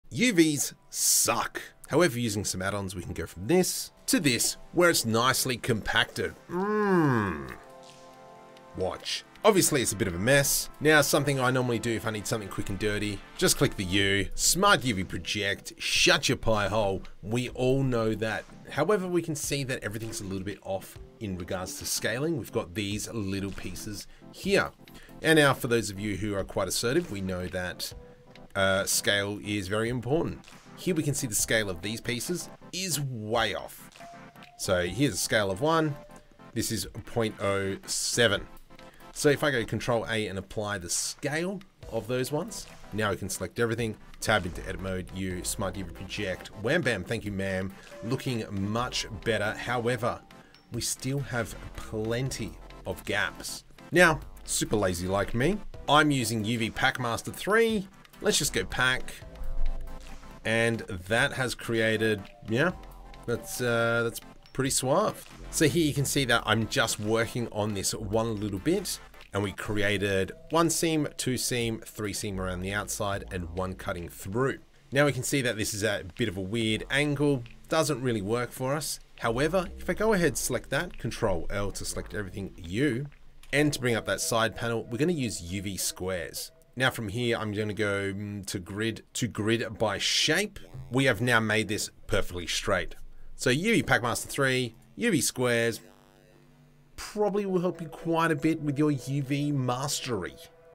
UV Editing Tips in Blender with Awesome ADDONS
We look into two Blender addons called UV Packmaster 3 and UV squares to help you clean up your UV maps. Just click on the links below will you?